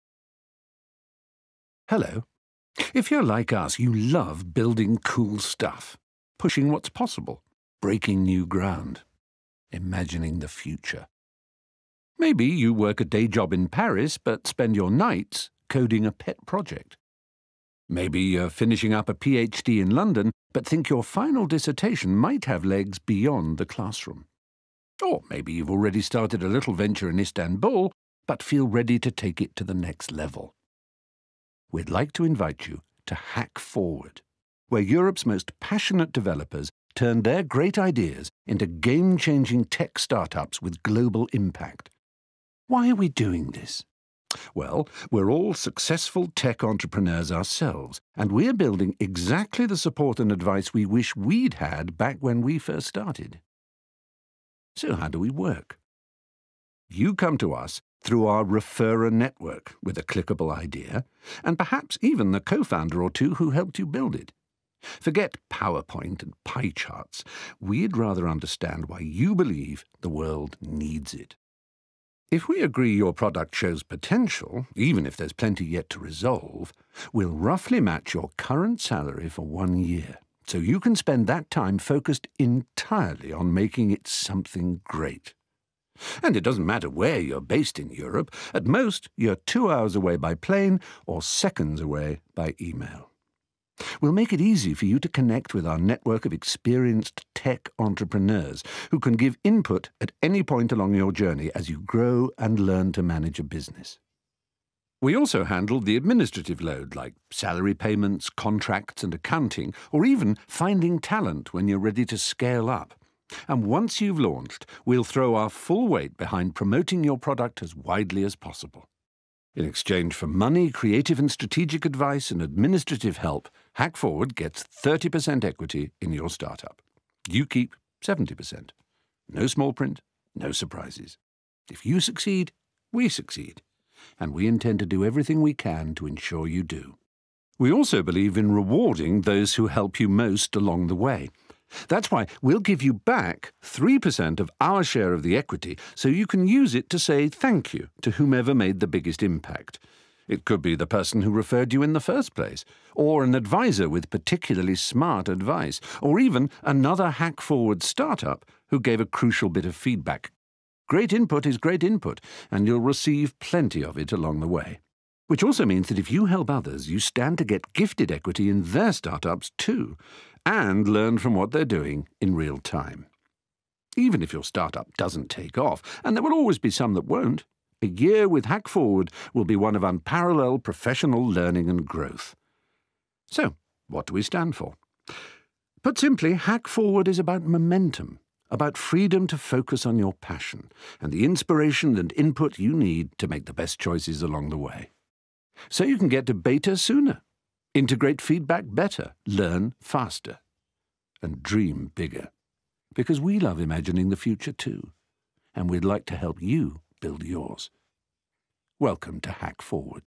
HackFwd
With HackFwd, top technology talents are free from their day jobs to build their own game-changing companies, and put European innovation on the global map. IDEO worked with Europe's "serial entrepreneur" Lars Hinrichs to design a new company that accelerates the route to beta, profitability and success by enabling individuals to focus on what they do best, and providing support on tasks that do not drive actual value.